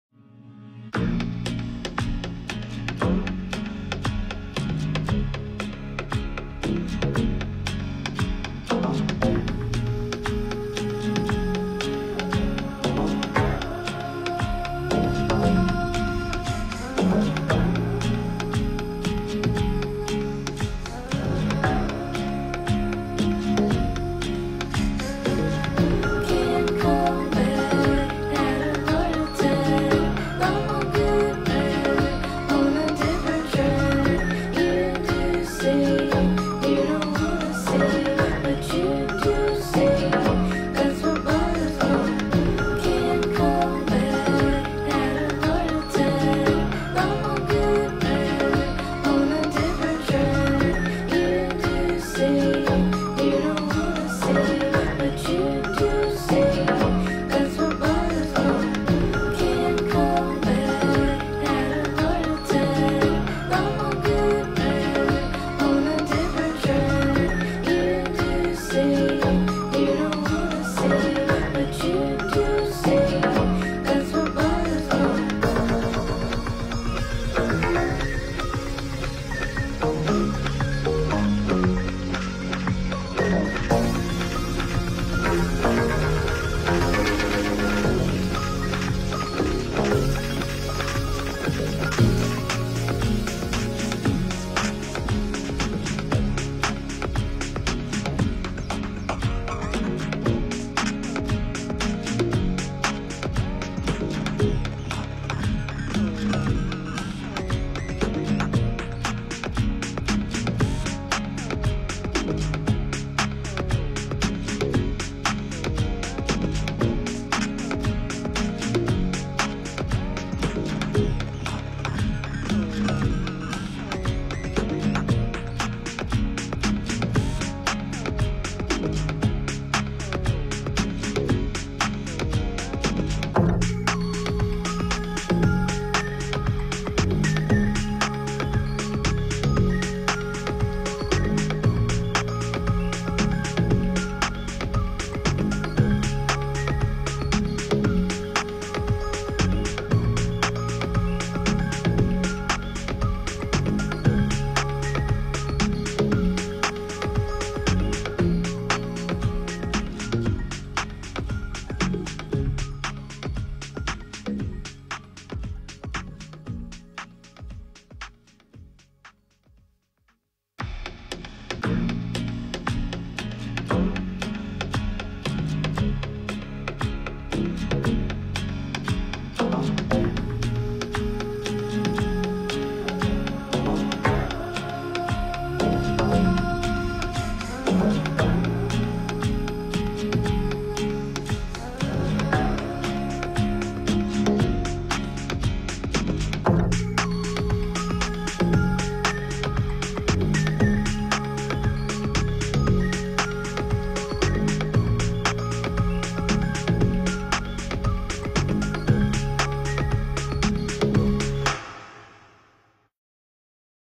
The Great Horn Antennae Drone Flight
This historic property in Holmdel NJ is where the big bang theory happened.